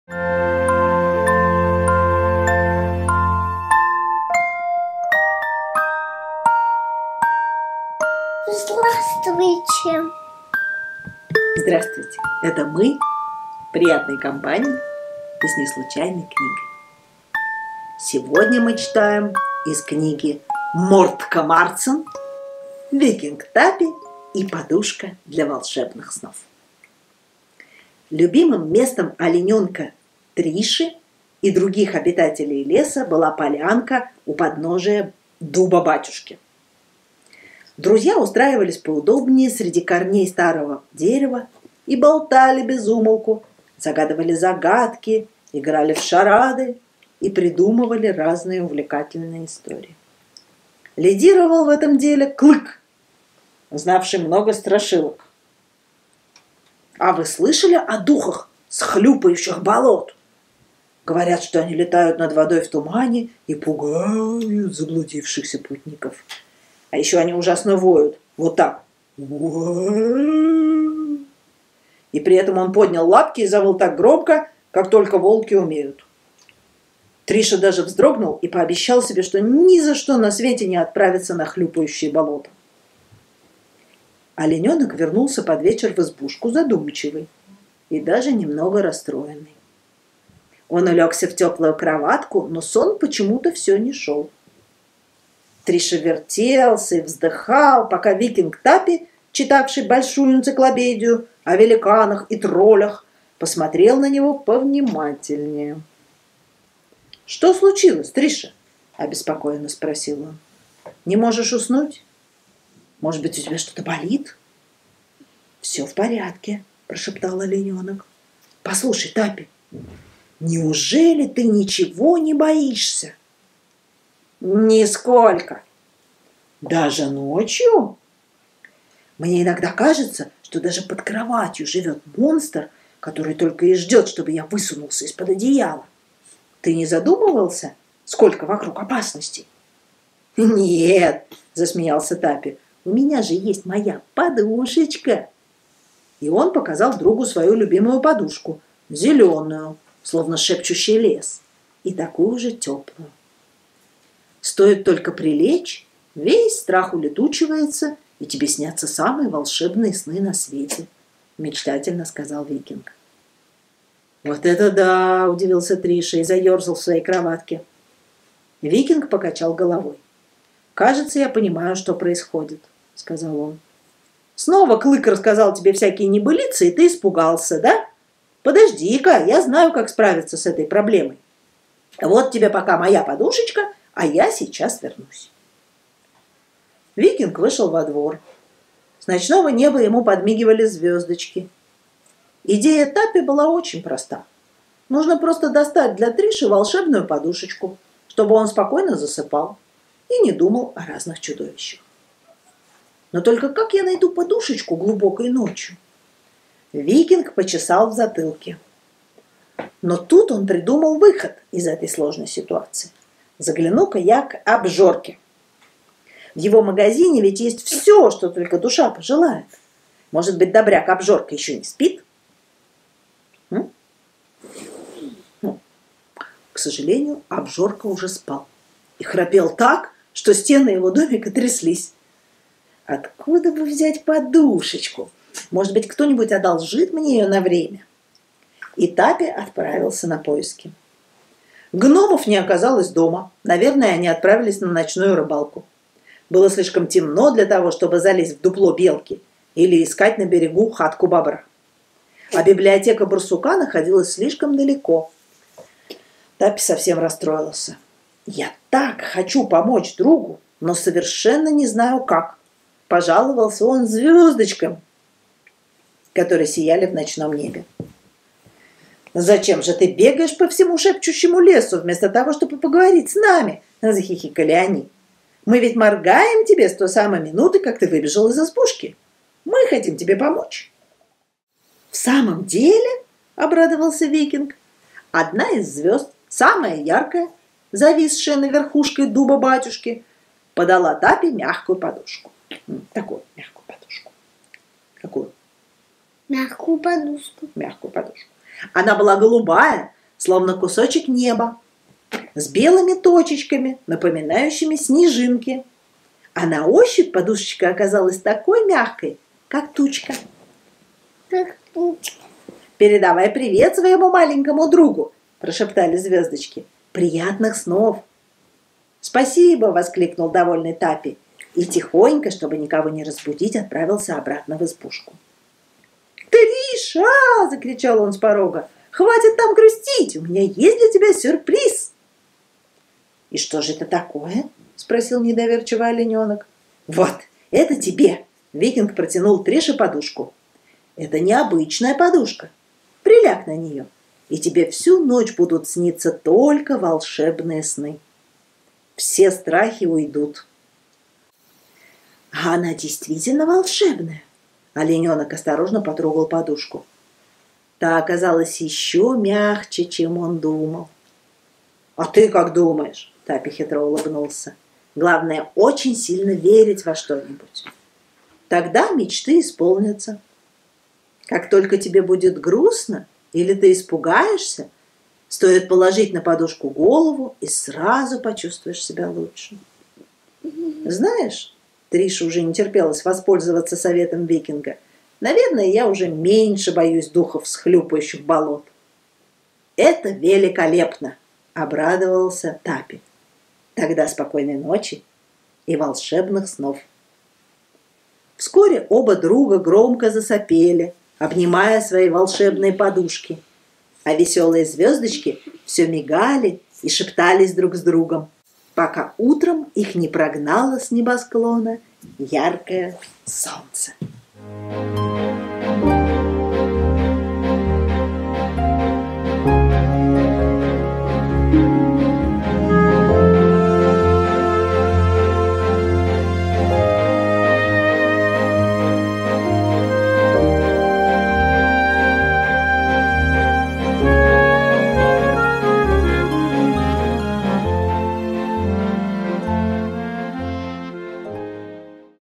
Лучшее Детям №15. "Викинг Таппи и подушка для волшебных снов" Марцин Мортка
Семейное чтение вслух детской книги. Выбор неслучайной детской литературы. Прочитай еще одну сказку на ночь. Из Большой книги приключений викинга Таппи
Очень уютно устроиться на диване и читать вместе с ребенком. Для вас и ваших детей от 4 лет звучит наша аудиосказка.